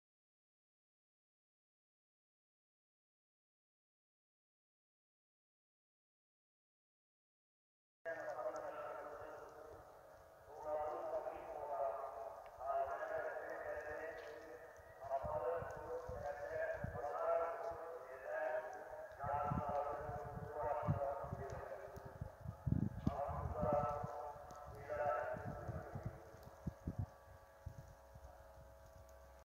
YILDIZELI# belediyesi deprem# bölgesine yardım başlattı yardım haberleri
belediye başkanımıza saygılar sunarız
bu örnek davranisindan dolayı